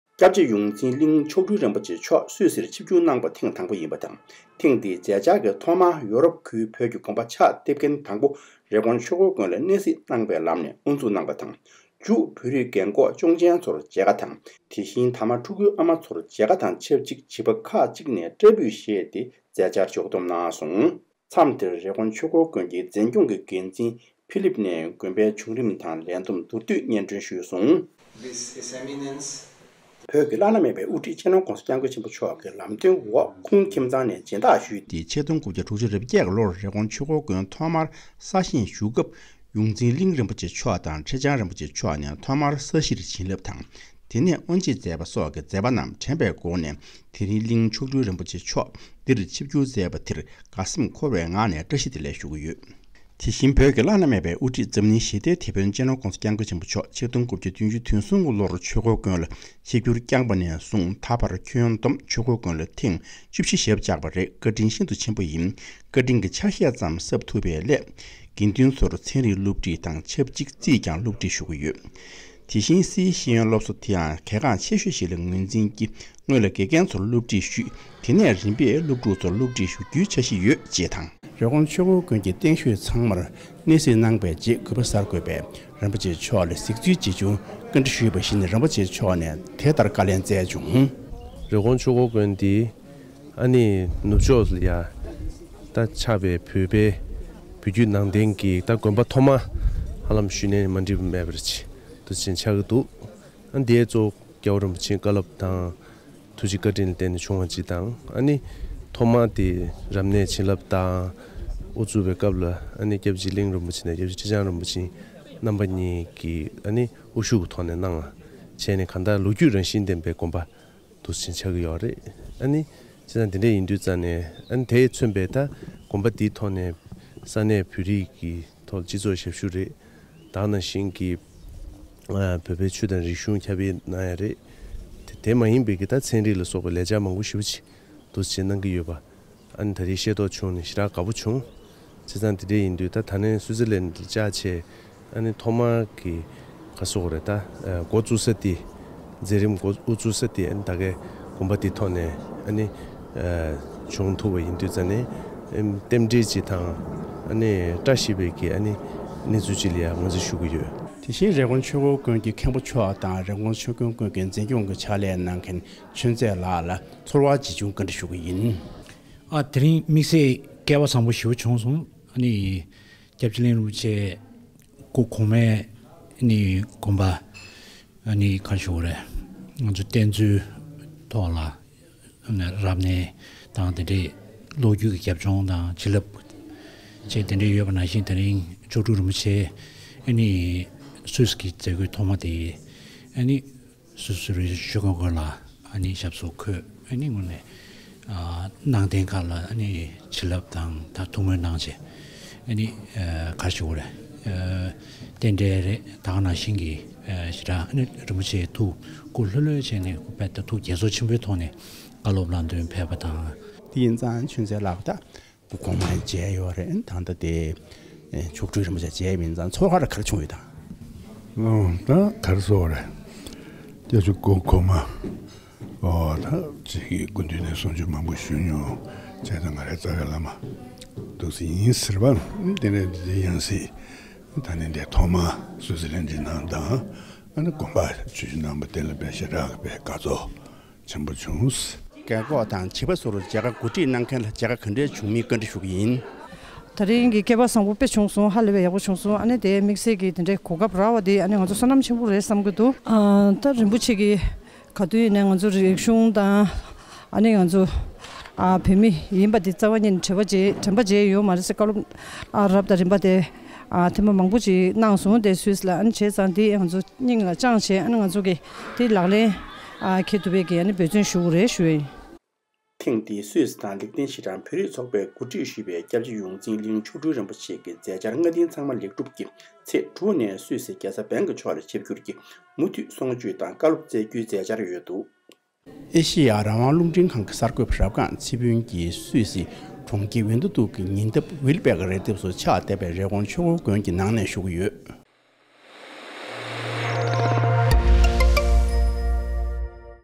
༸སྐྱབས་རྗེ་གླིང་མཆོག་སྤྲུལ་རིན་པོ་ཆེའི་སུད་སི་ནང་གི་ཆིབས་བསྒྱུར། HE Ling Rinpoche's Swiss visit
༧སྐྱབས་རྗེ་ཡོངས་འཛིན་གླིང་མཆོག་སྤྲུལ་རིན་པོ་ཆེ་མཆོག་གིས་སུད་སི་ནང་ Jona དང Ruti ཁུལ་གྱི་བོད་རིགས་རྒན་ཁོག་དང་།  གཅོང་ཅན་ཚོར་མཇལ་ཁ་བཀའ་སློབ་གནང་ཡོད་པ་རེད།
His Eminence Ling Rinpoche met with Tibetans of all age residing in Rapperwil and Ruti Town in Switzerland, gave blessing to the elders and guidance to the younger Tibetans
Reporter & Producer: Rabgang Tsering Phuntsok